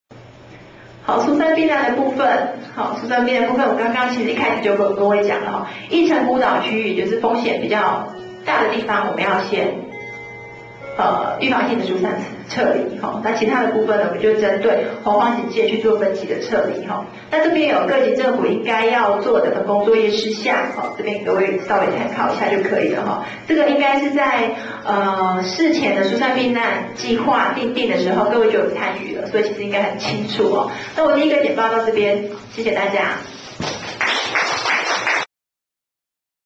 102年度土石災害防救業務講習_土石流災害管理與警戒值介紹何謹余
2013 / 05 / 02
102年度土石災害防救業務講習
1.介紹目前水保局對於土石流災害管理之方法。 2.講述行政院頒佈之疏散撤離分工命令，地方應如何協同中央一同面對土石流災害的發生。